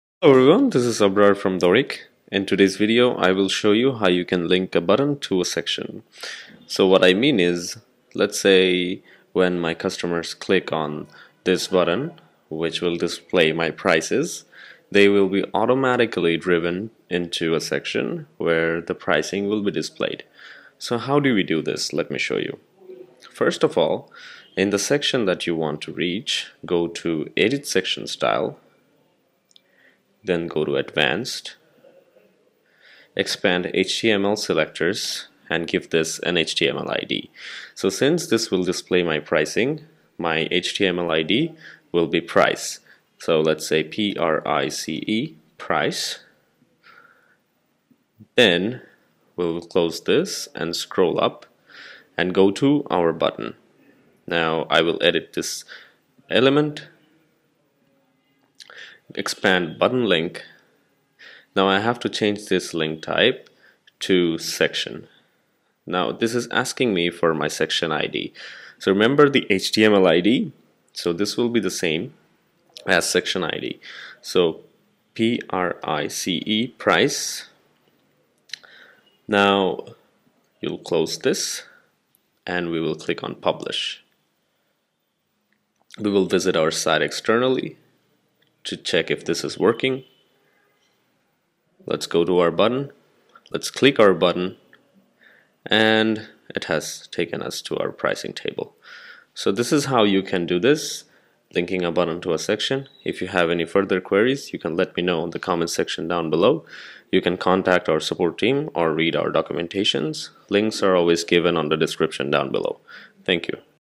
How to Link a Button to a Section on Your Website
Linking a button to a specific section on a webpage is a popular use-case of websites and here from this video, you will learn - how to Link a Button to a Section on your website on Dorik.

The process is quite simple. You have to give an HTML ID to the section and then you have to call the given id in the Button Linking option.

Please check out the video to learn the step by step process, but if you got any further questions, please feel free to reach our customer support team